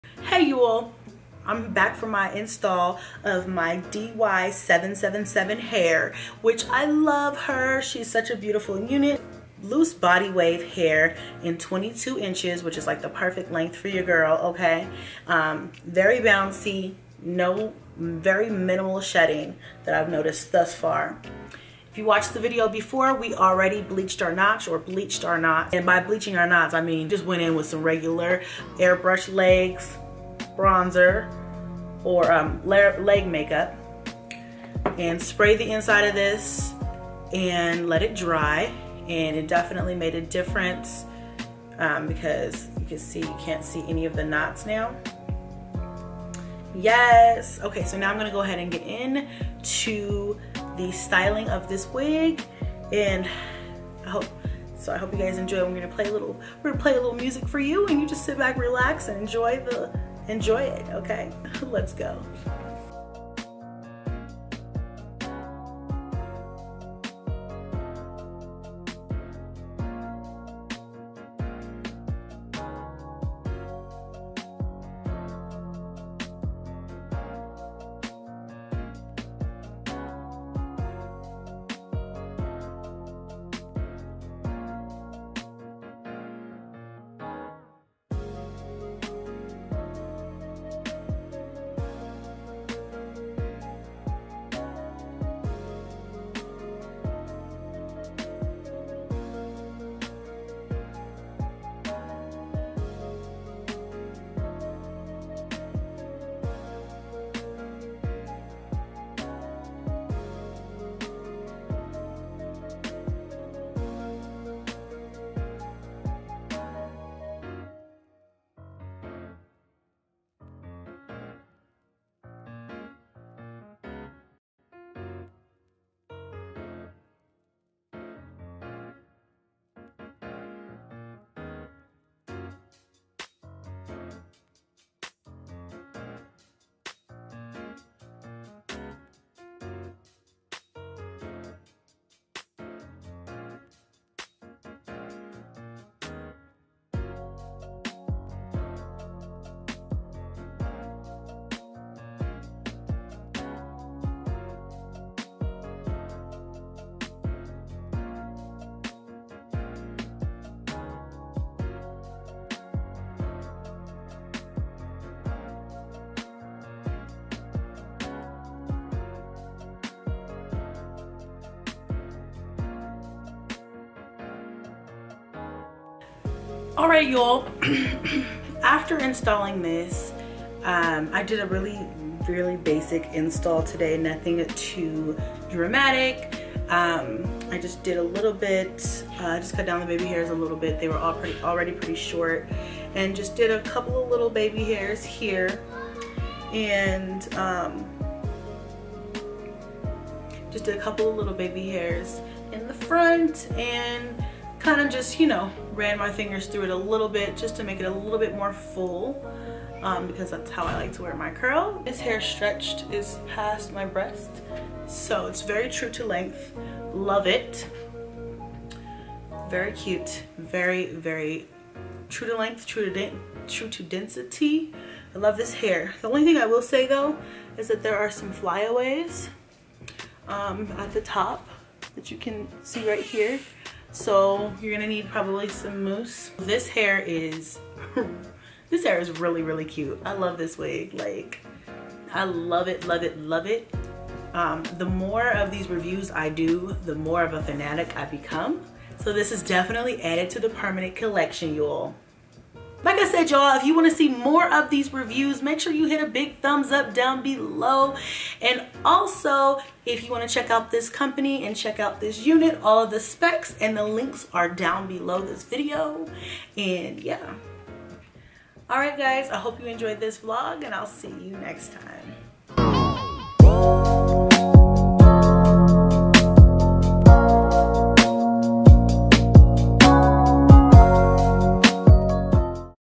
WATCH ME SLAY THIS UNIT FT. DYHAIR777
Hey Loves!! 😘-xoxo Enjoy

Wig Info: Malaysian Lace Frontal Wig Pre Plucked Loose Wave 130% Density 360 Lace Wig 22 inch

🔴JOIN MY FACEBOOK MOMMY CHAT GROUP🔴
          MELANIN MAMAS